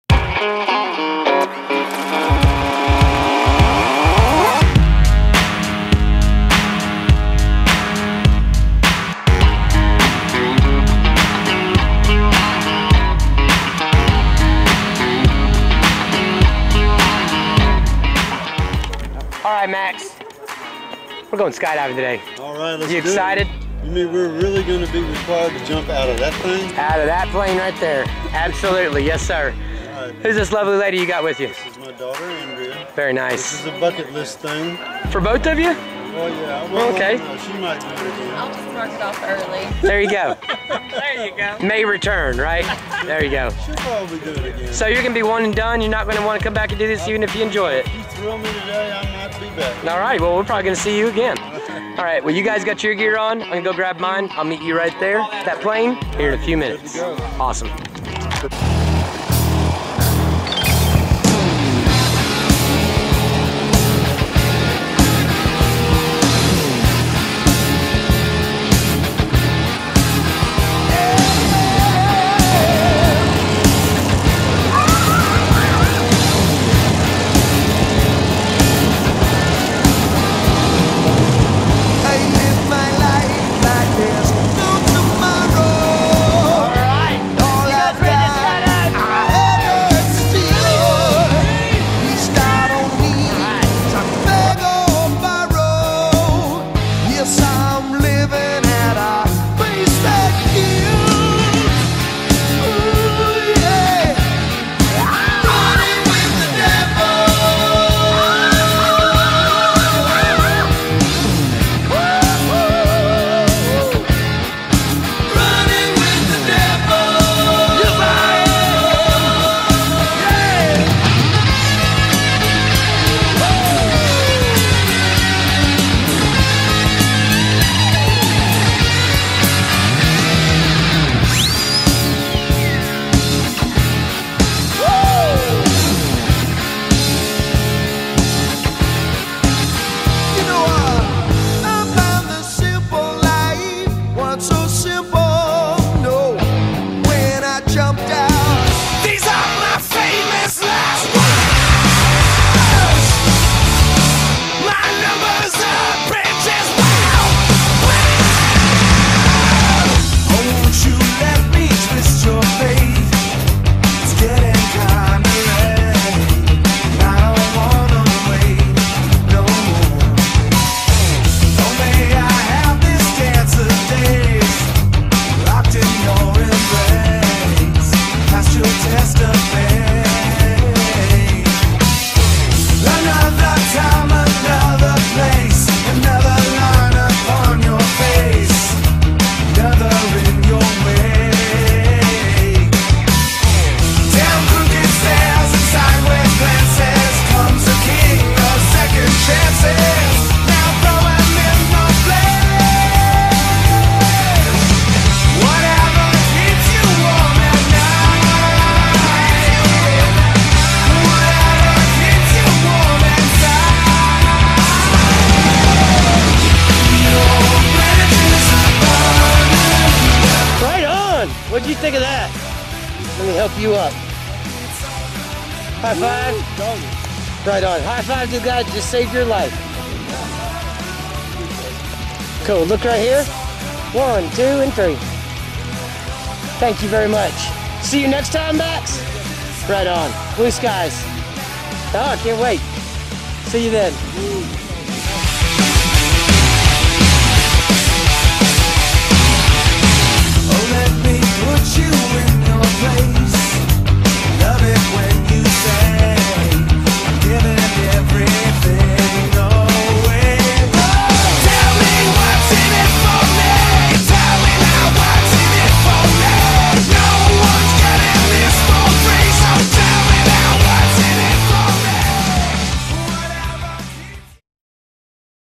Norman Max Sanders Sky Diving with daughter 2012
Another "bucket list" item taken care of.  Got to go with daughter.  Great thrill...highly recommended.  Don't leave home without a good pillow case and some string (helps to break your fall).  Don't forget to check out